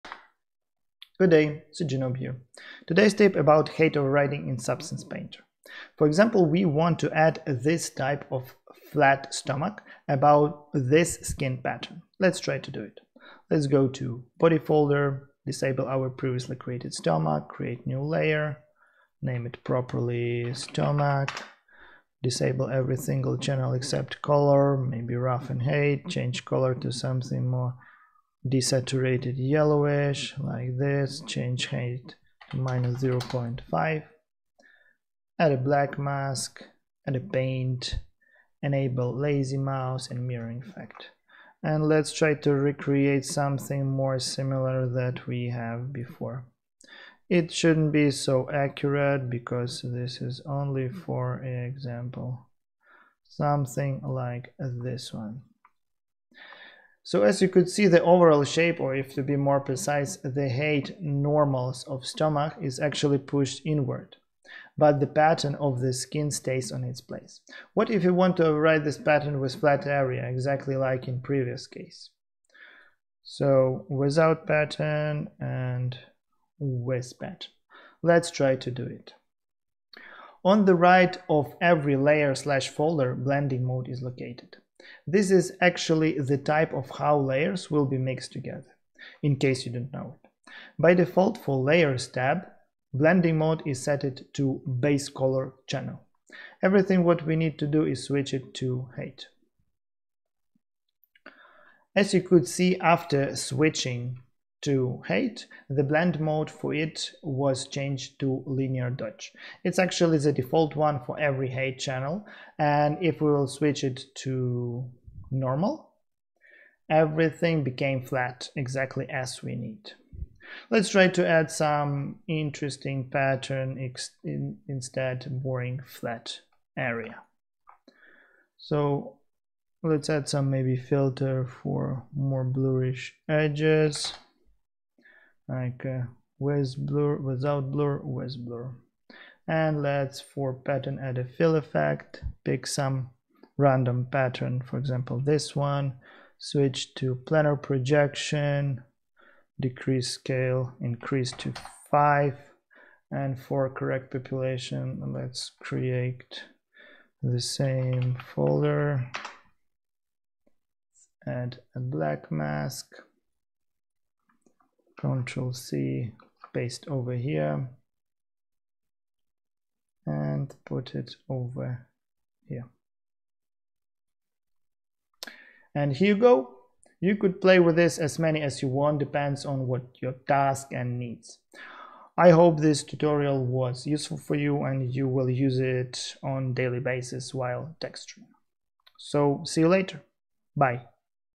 Height Override in Substance Painter
The easiest way how to override Height in Substance Painter
I  hope It'll be useful for you guys